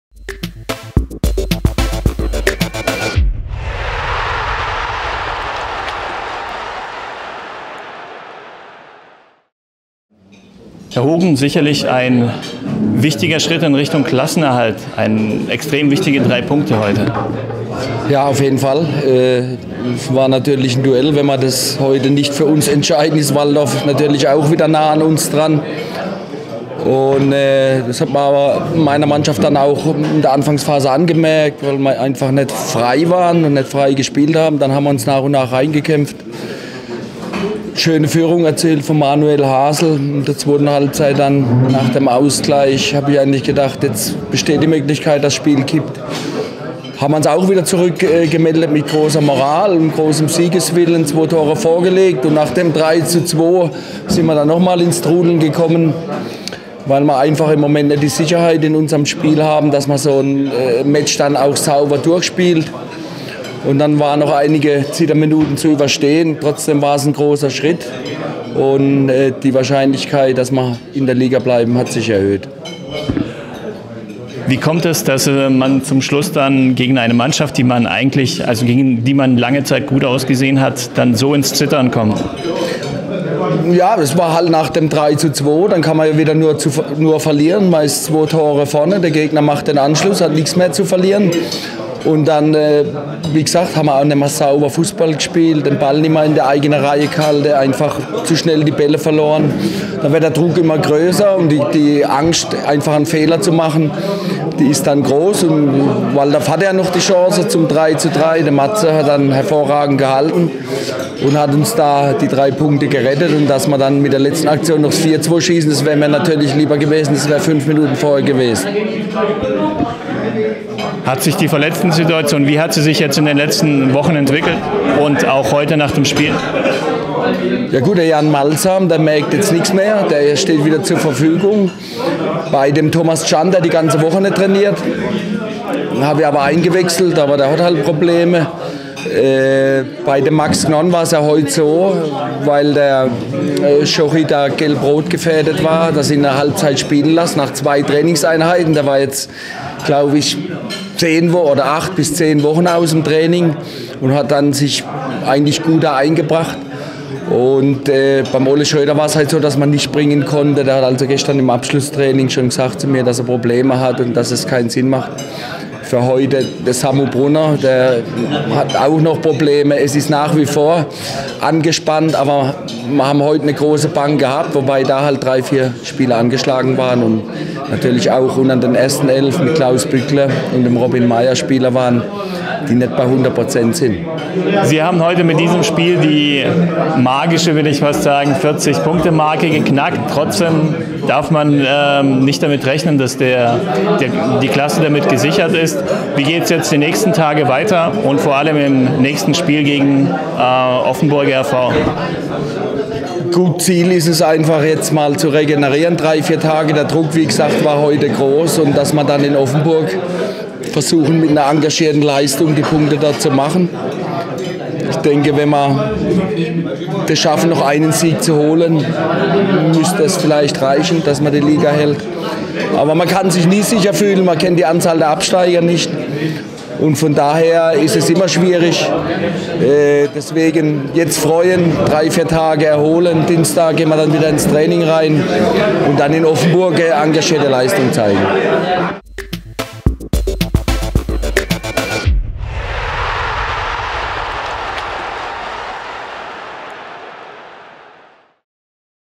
Peter Hogen - SV Spielberg - zum Spiel vs. FC-Astoria Walldorf II, im FuPa.tv-Interview am 28.4.2017
„Es war ein großer Schritt.“

Peter Hogen, Chef-Trainer des SV Spielberg, im FuPa.tv-Interview nach der Begegnung SV Spielberg - FC-Astoria Walldorf II am 31. Spieltag der Oberliga Baden-Württemberg 2016/17.